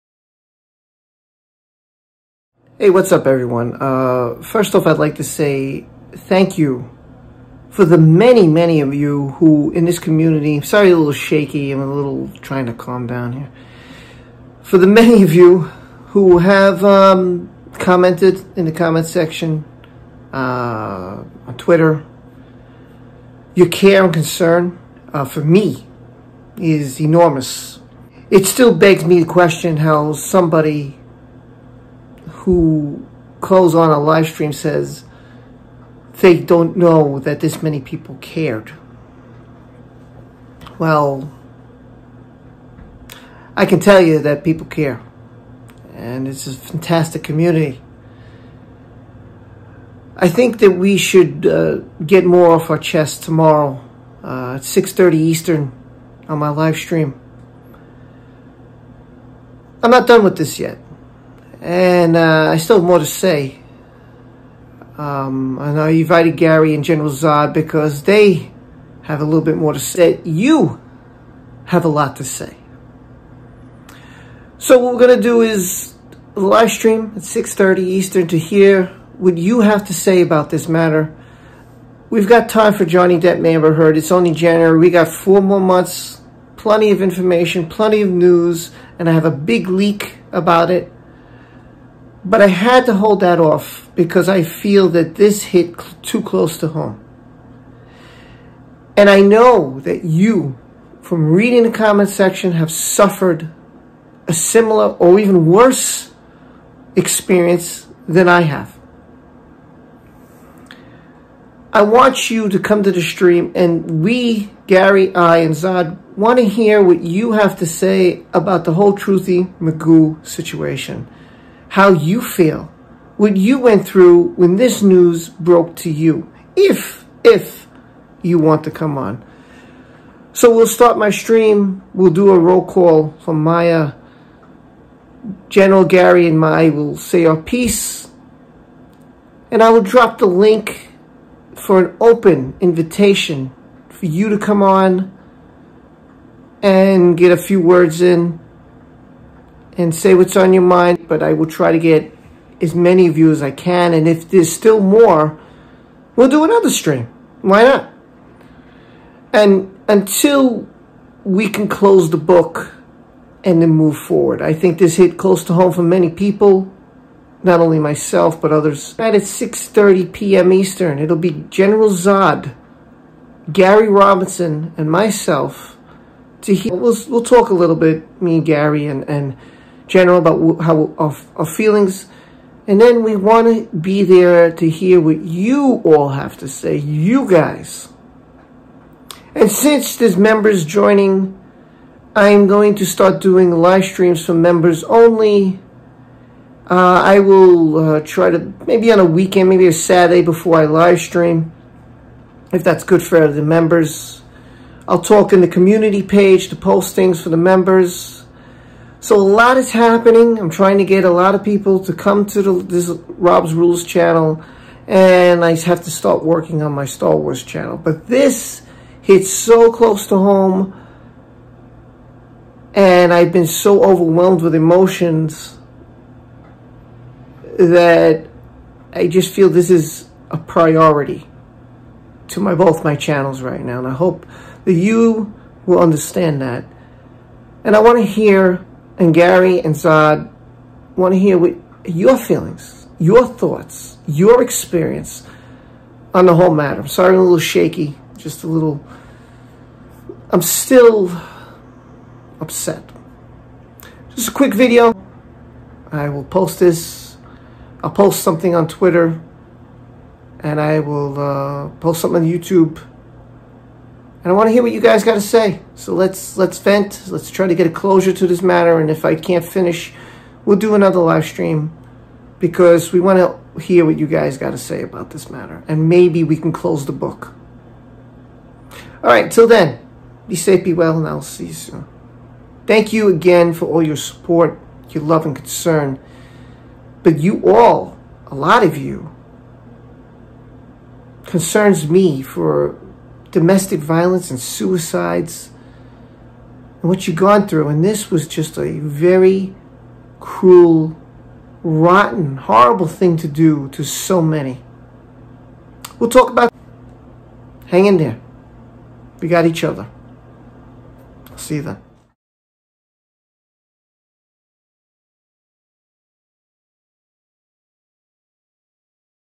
🔴 OPEN Livestream ! Getting Closure and Venting About The Truthy McGoo Situation
At 6:30 PM Est Monday Night Gary Robinson and General Zod join me as we get YOUR feelings and thoughts with an OPEN livestream to discuss the Truthy McGoo Situation. We need a closure and vent. We can do that together.